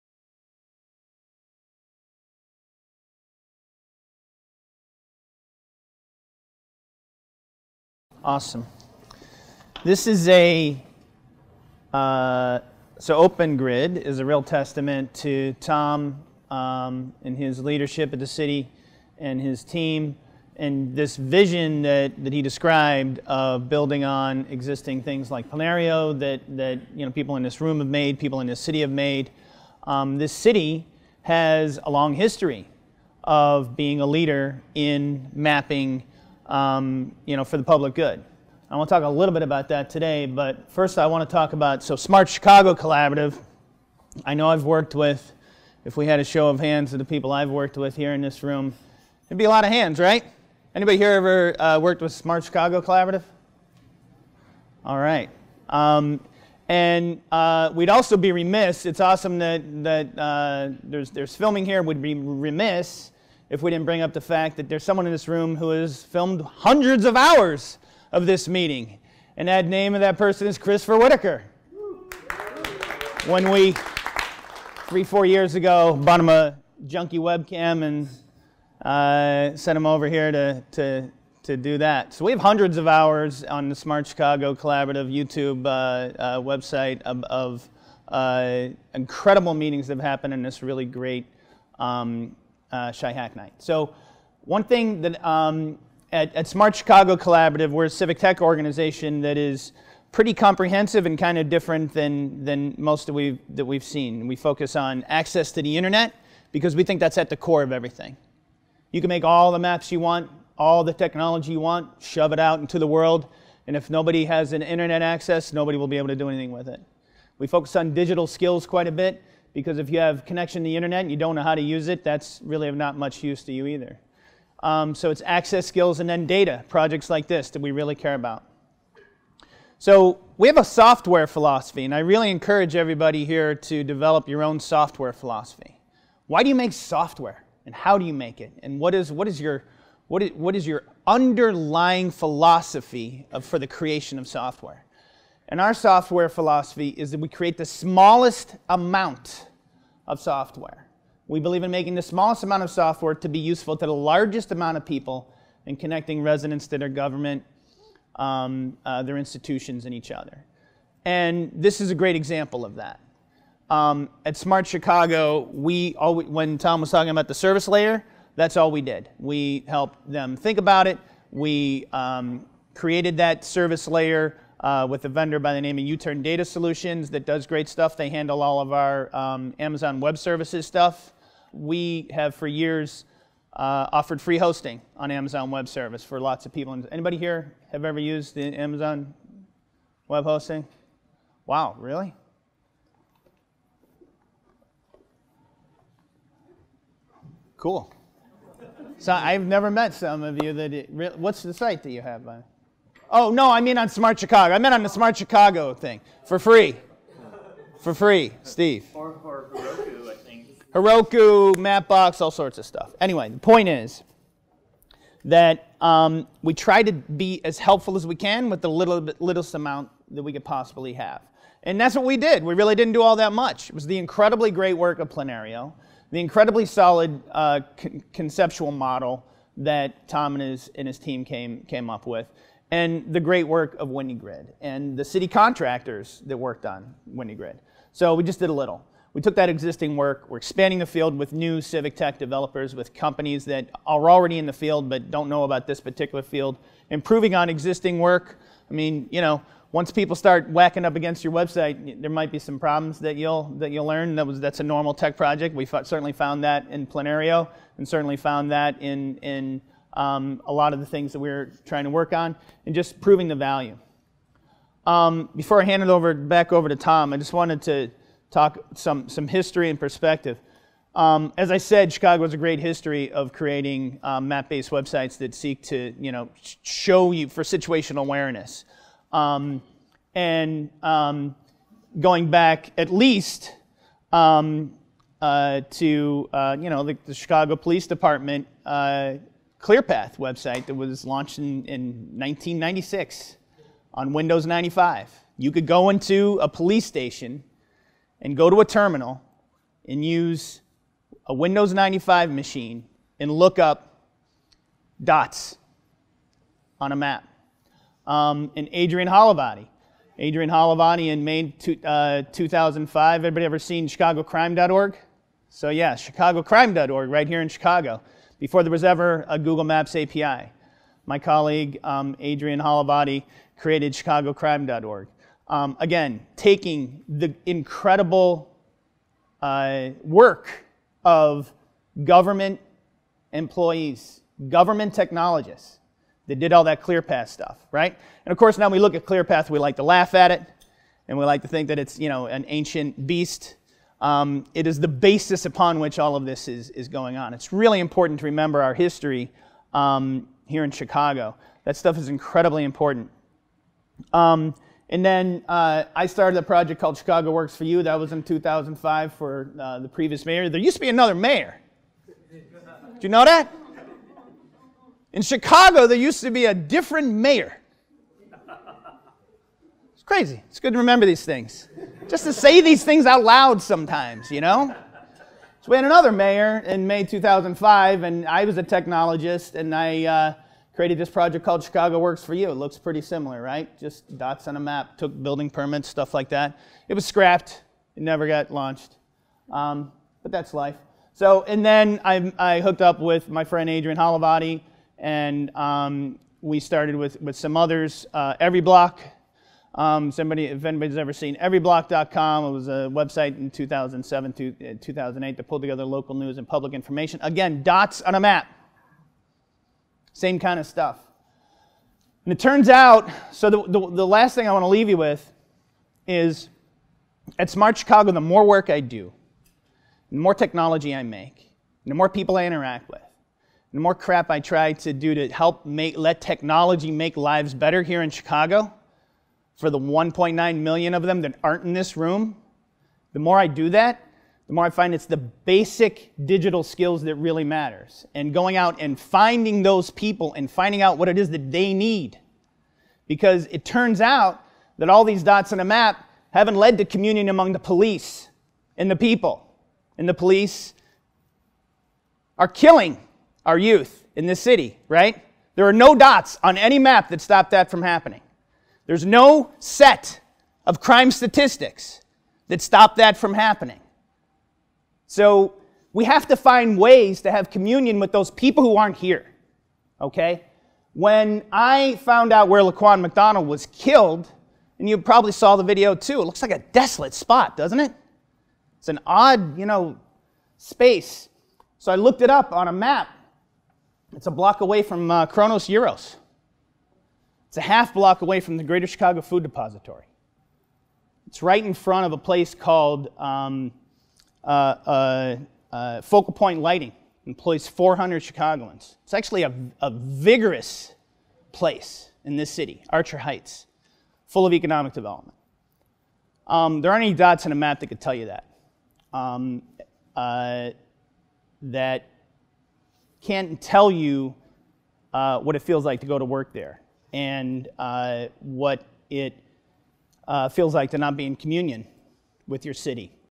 The Inefficacy of Dots on Maps
This is a snip from my talk at Chi Hack Night about how dots on maps have never led to communion in Chicago.